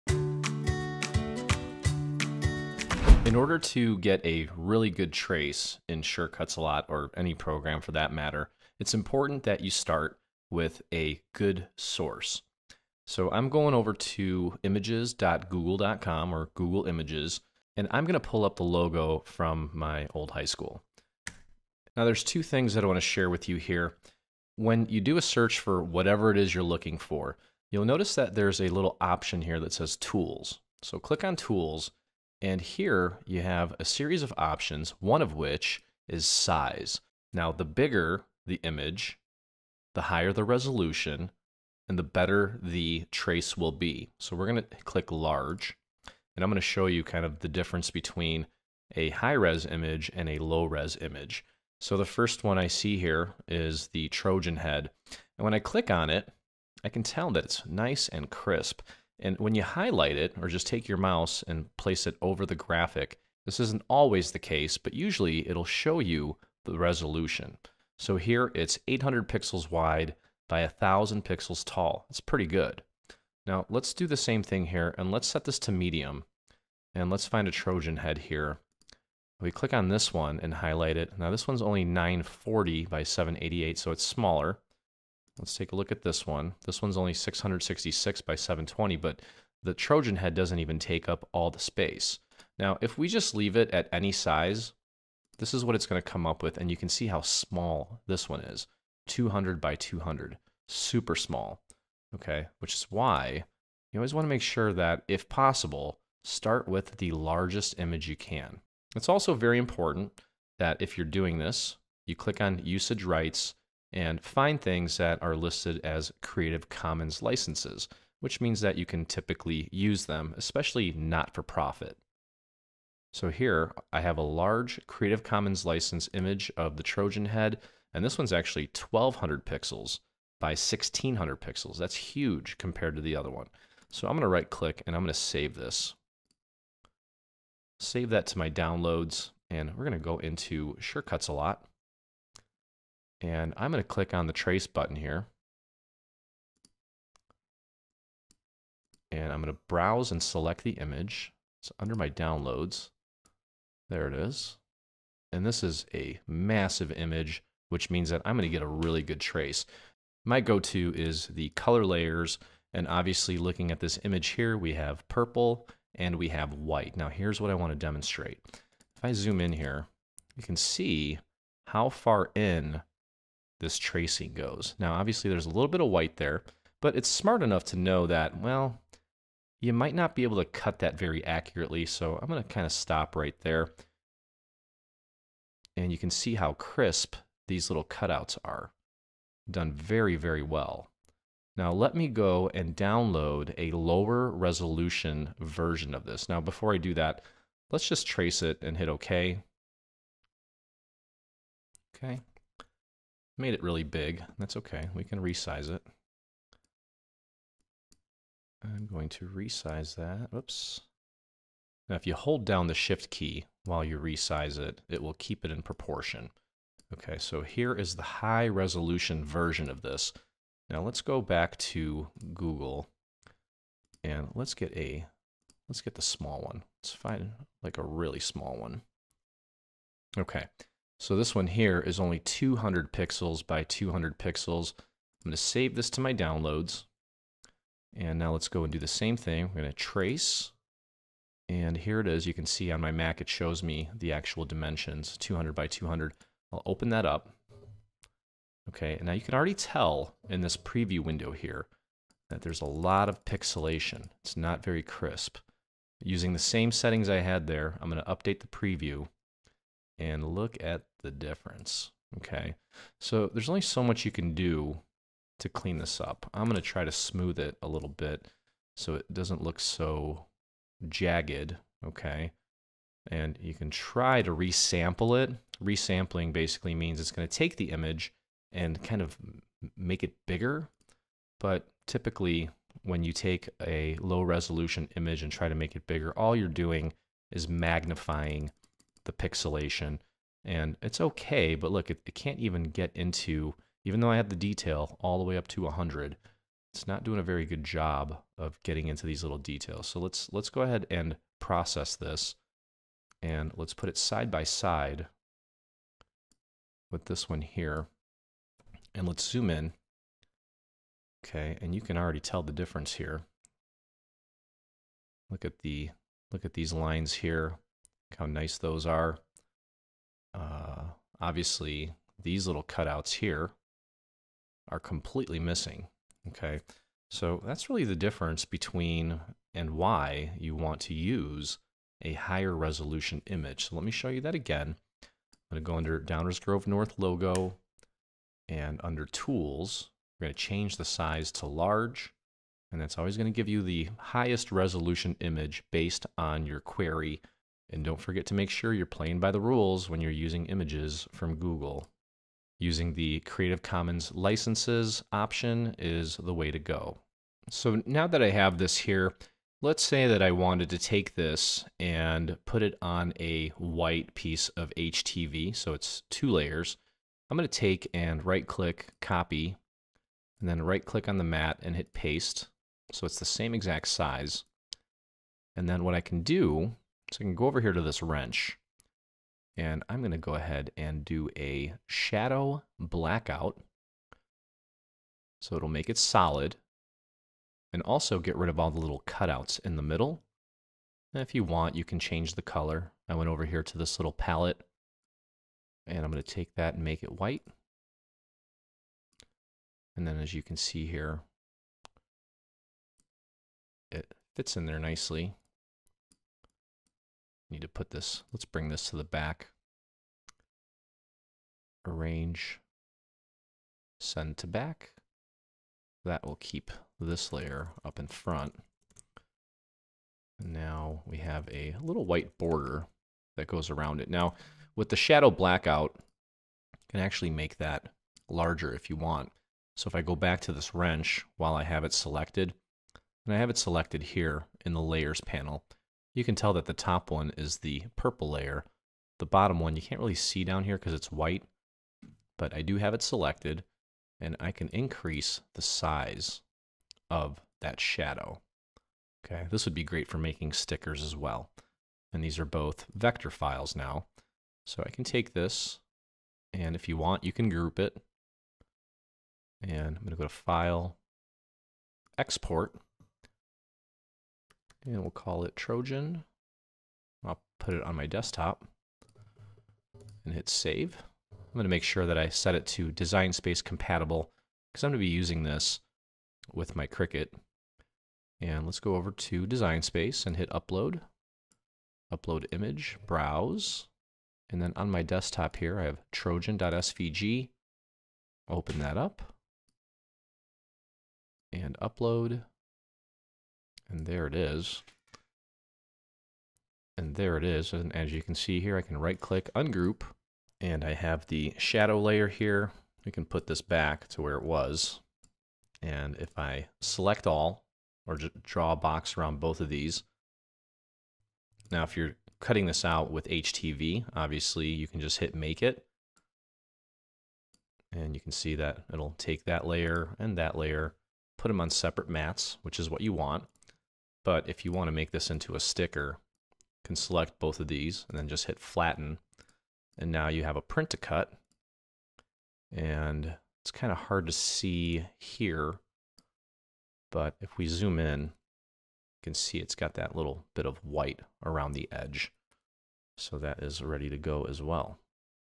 Tracing Google Images - Best Practices for Sure Cuts A Lot and Cricut Design Space
Unlimited Dreaming Tree projects now on Creative Fabrica

In this video, I discuss best practices for achieving perfect tracing of online (Google) images using Sure Cuts A Lot. I also discuss how to convert your tracing into stickers. I also show how to import the files into Cricut Design Space.

Sure Cuts A Lot is NOT a subscription based program. Buy it once and forget the monthly charge.